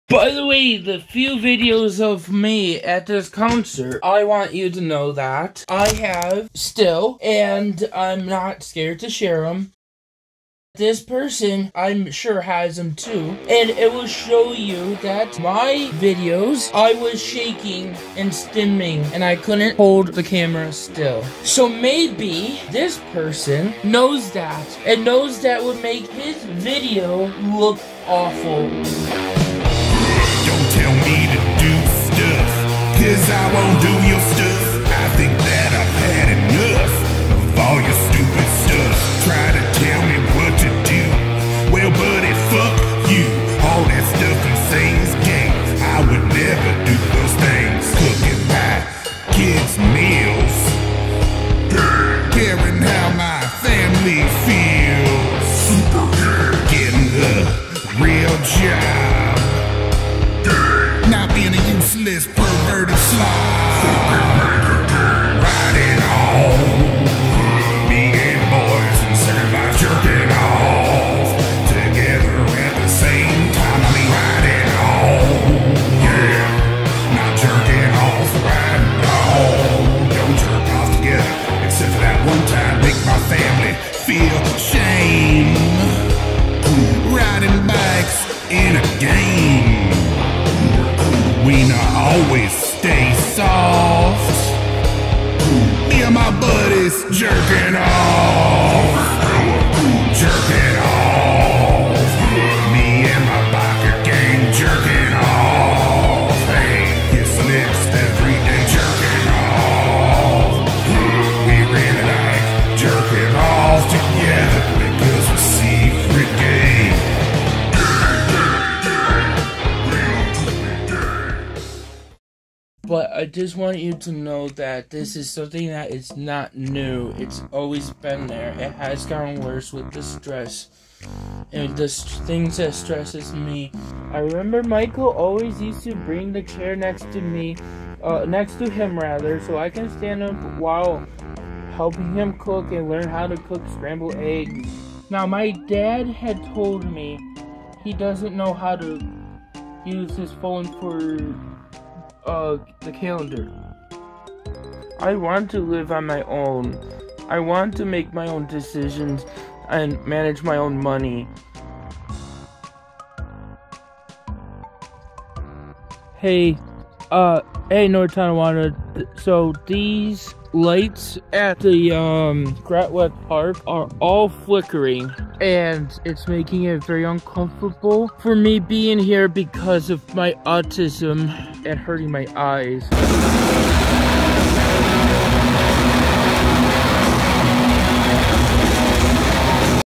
Addressing Andrew Ditch's Lies About the Concert
These are the videos Andrew Ditch doesn't want you to see. In this video we highlight one of the many mistakes Andrew made in his long rebuttal to my concert video in which Andrew missed the point entirely. One of Andrew's counter points is that he was "stimming" during the entire concert and that I intentionally did not include those videos of him at the concert because it would make me look bad. The truth is simply that the videos were long and boring and irrelevant outside of proving that Andrew went to the concert. The videos he referenced were linked in the description of the original video and anyone can click on them and view his full videos of him at the concert.

Andrew attributes rocking side to side as a sort of "stim" and proof that he is autistic. He seems unaware that normal people shift their weight between legs all the time. If Andrew feels the need to rock back and forth a lot this has more to do with the fact that his legs are very weak from lack of use and having to support a 300+ lb. adult. This is also very strange because there are plenty of videos where Andrew is standing completely still, even this year. Does he just forget the act sometimes? Certain brain pills making him hyperactive? We may never truly know.